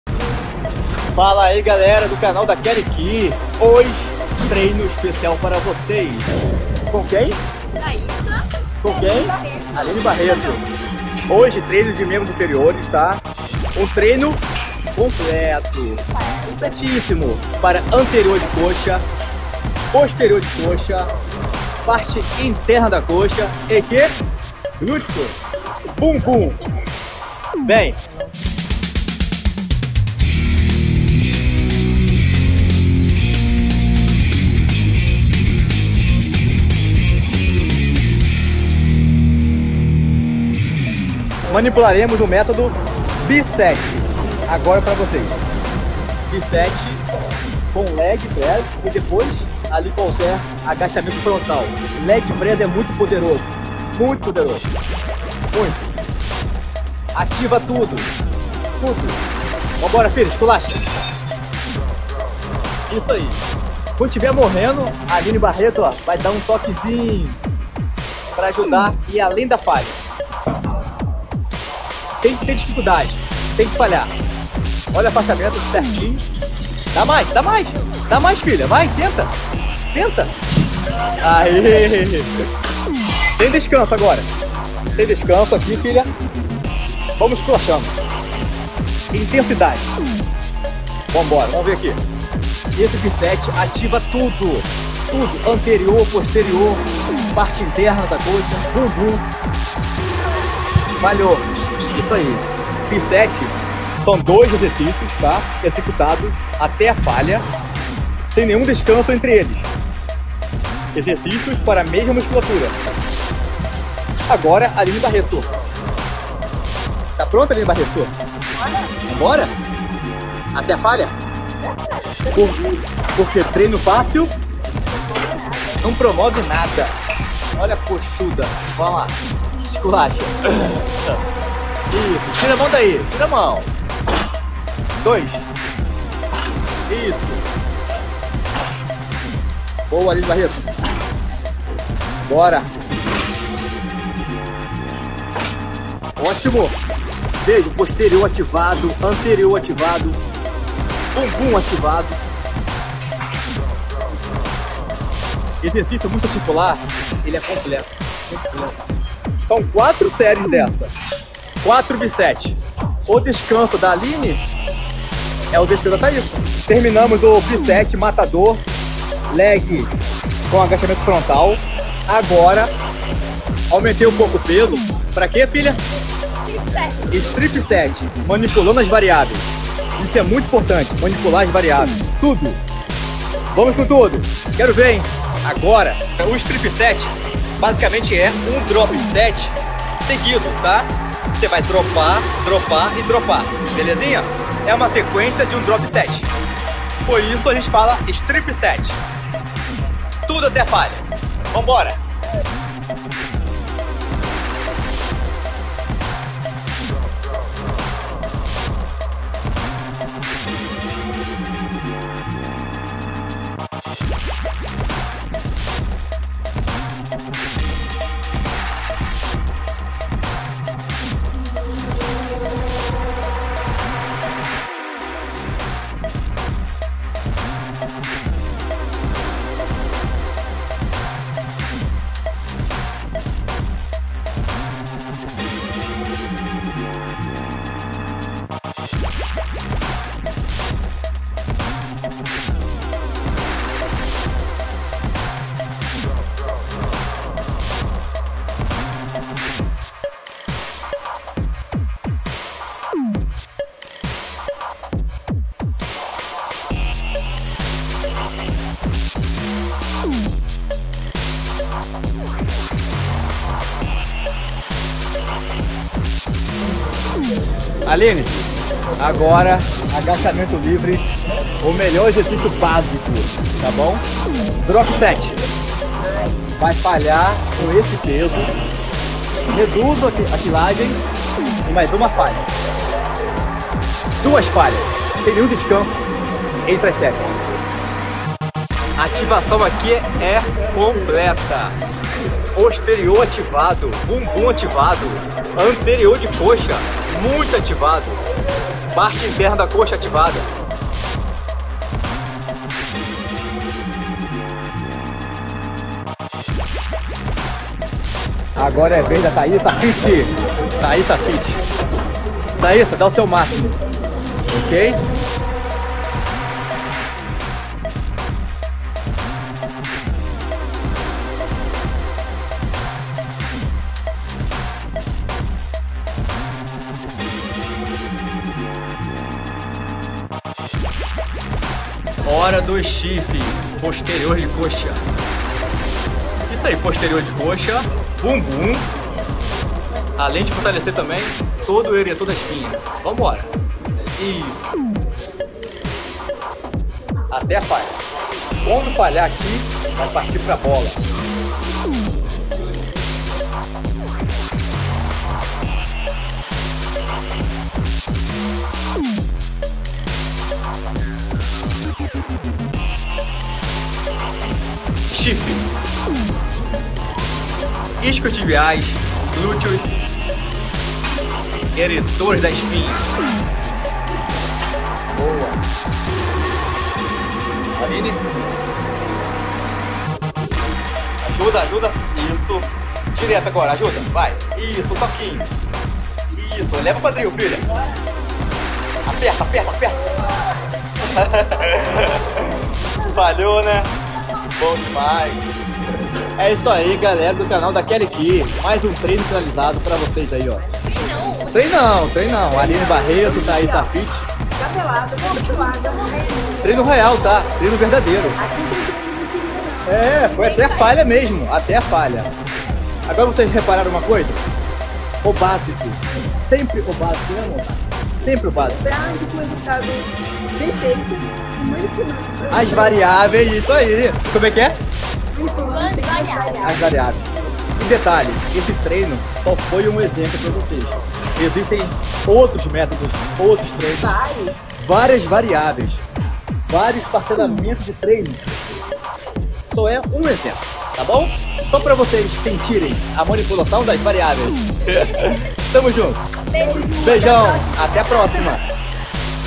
TREINO com a FAMÍLIA FITNESS [Treinador KAKA] | PROJETO BABA BABY
Hoje o Treinador Kaka invadiu o PBB e vai fazer um treino especial pra gente. Curtiram?